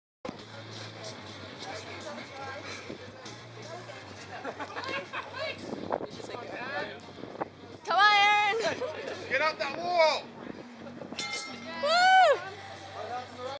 The Rhapsody of the Seas Rock Climbing
This is how we rock climb aboard the Rhapsody of the Seas